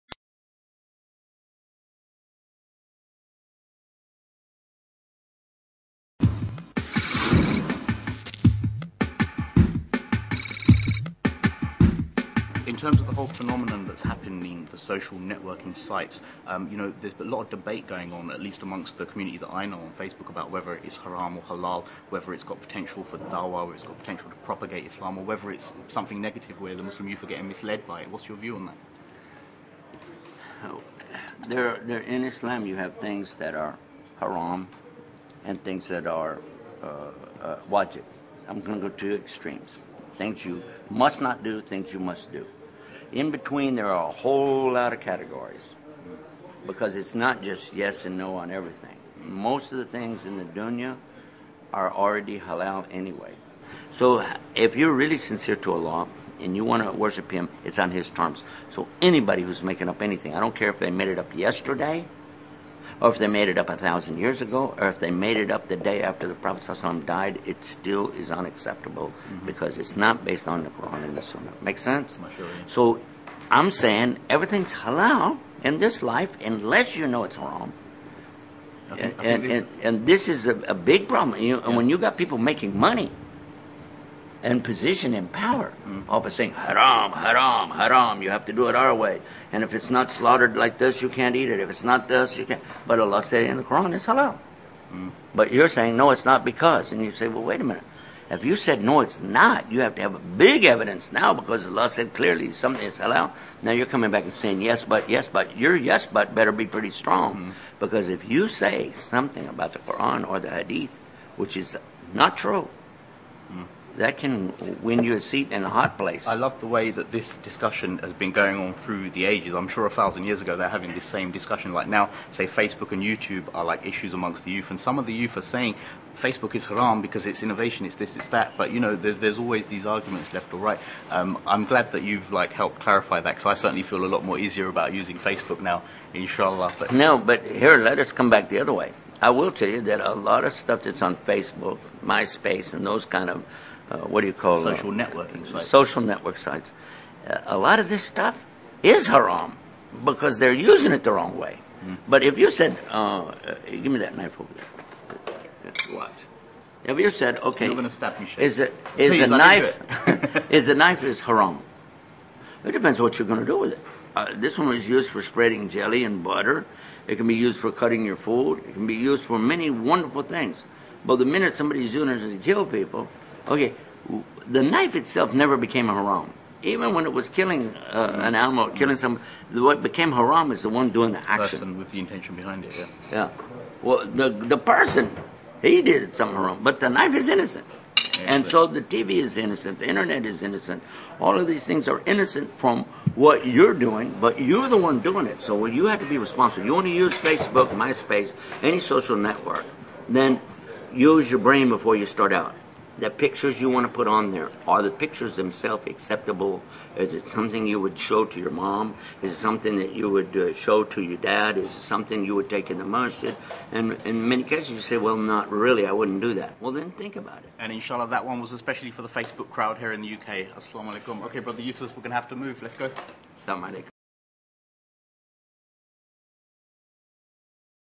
Facebook - Halal or Haram
by Yusuf Estes for those youth it is all about intentions for example the Knife